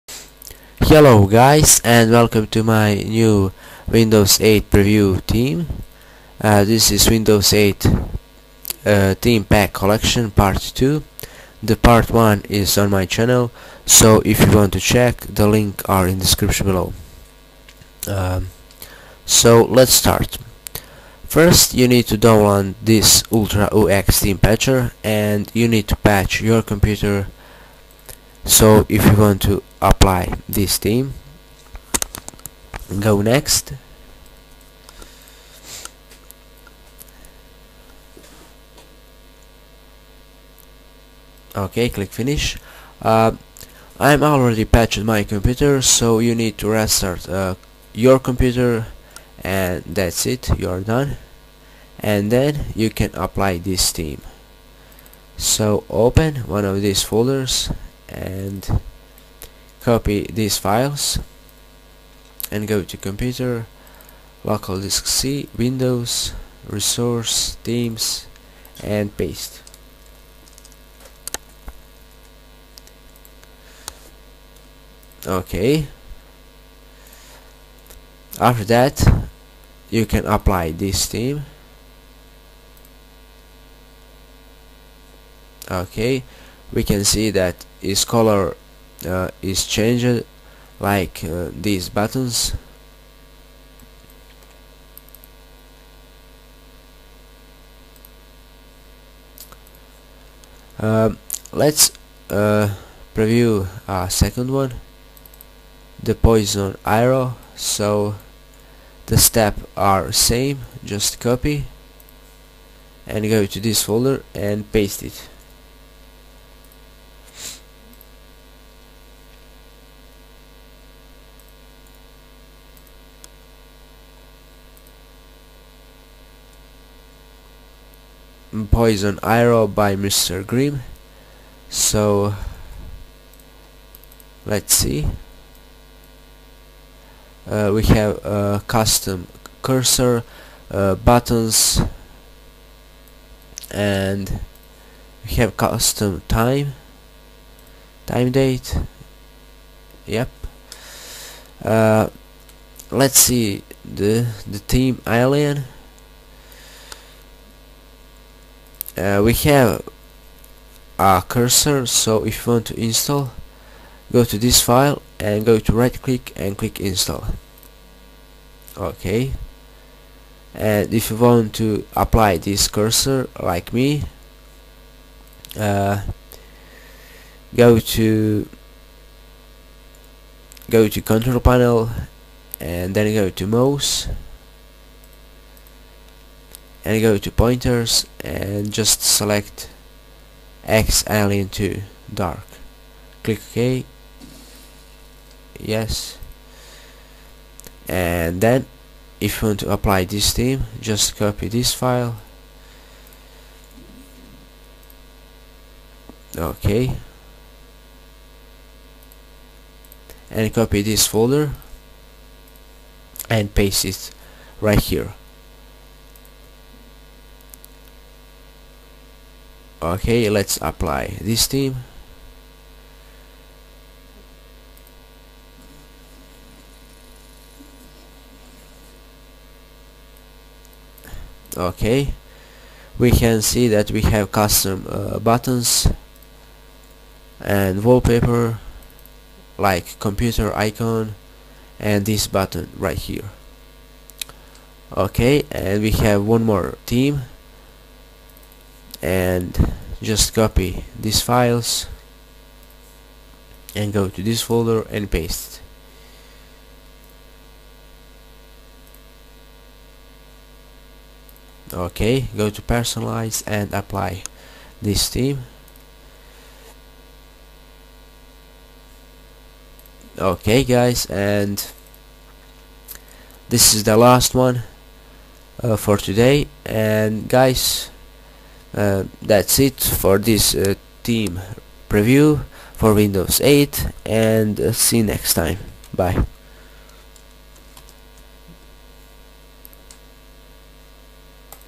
Windows 8 Theme Pack Collection Part 2
Here is my new video theme pack for windows 8 part 2.In this video I'm reviewing a three different theme for windows 8.